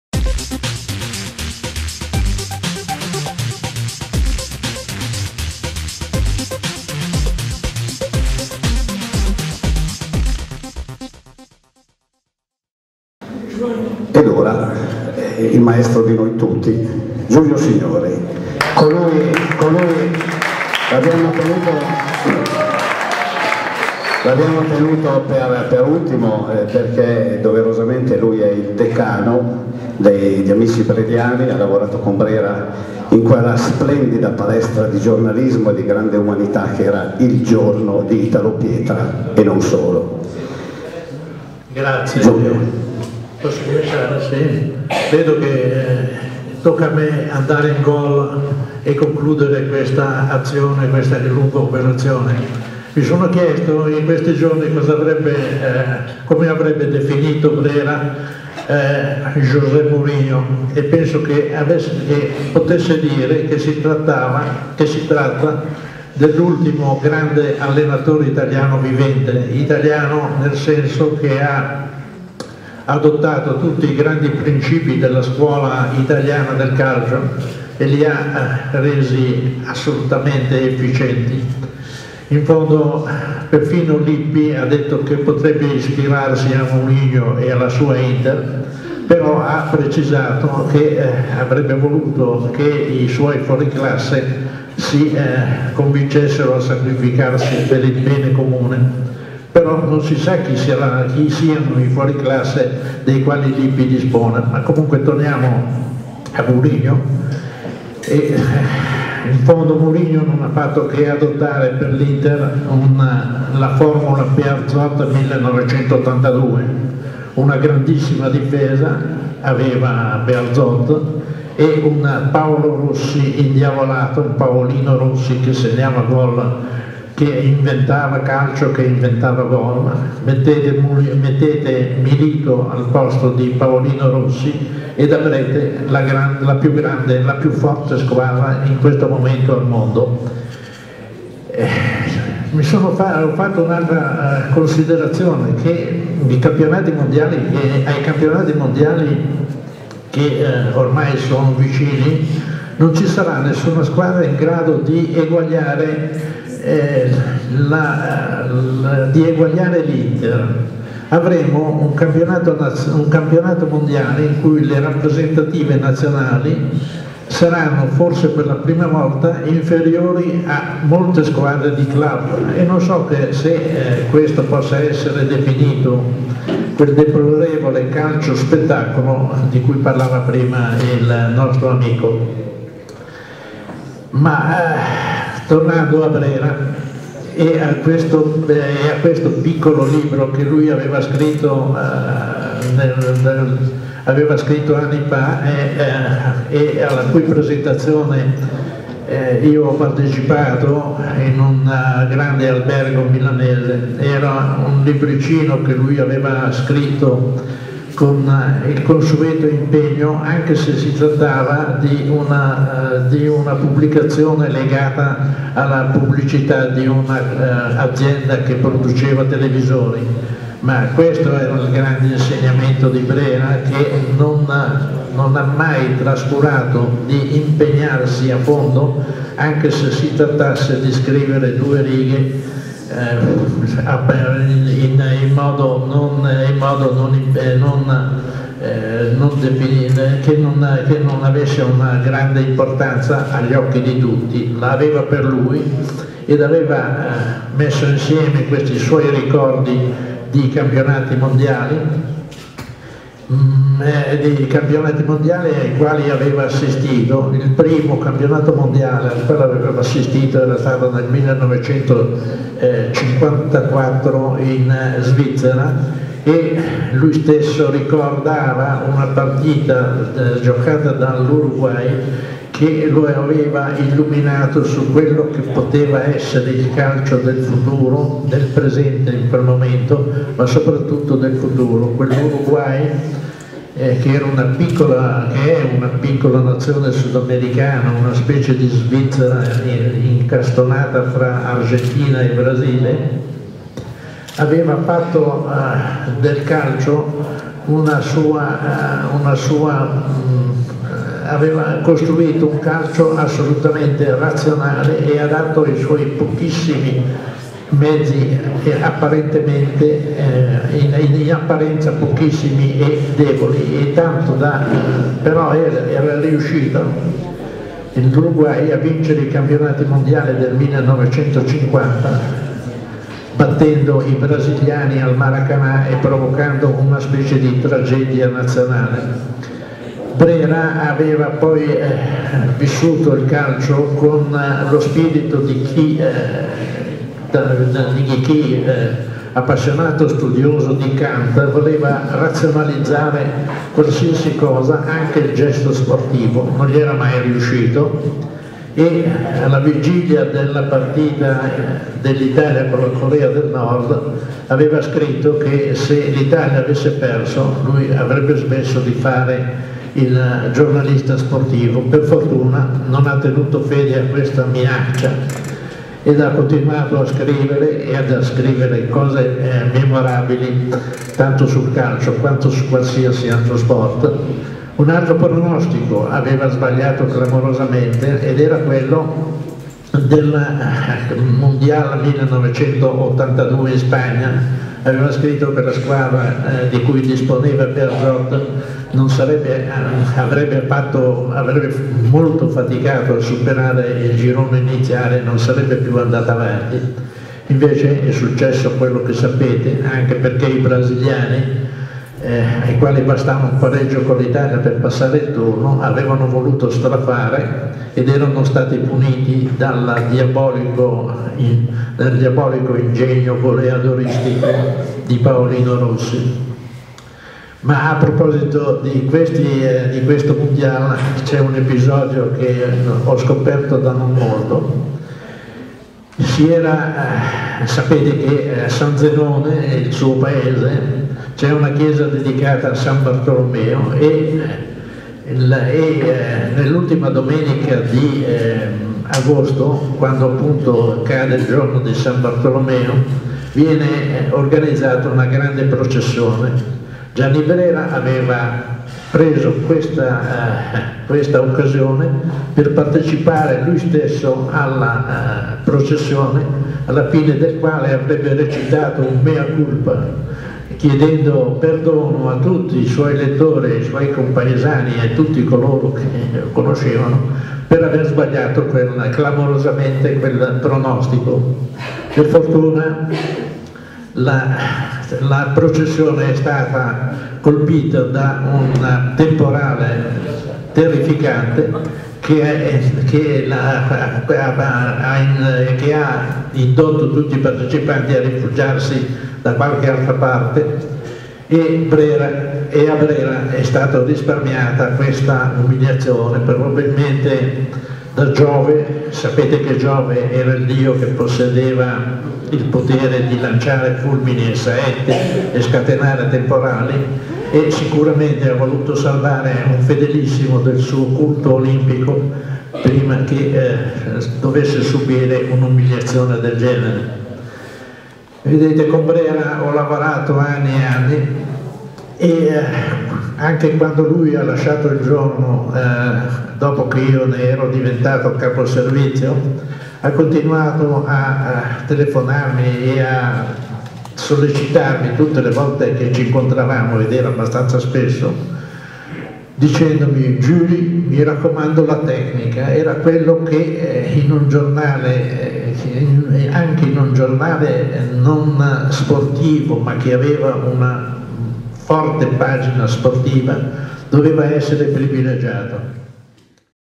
I Mondiali di Brera (6).
Incontro sul libro "La leggenda dei Mondiali" di Gianni Brera.
Sesta parte: intervento di Giulio Signori, giornalista de "Il Giorno".

Biblioteca Civica di Sannazzaro, 27 maggio 2010.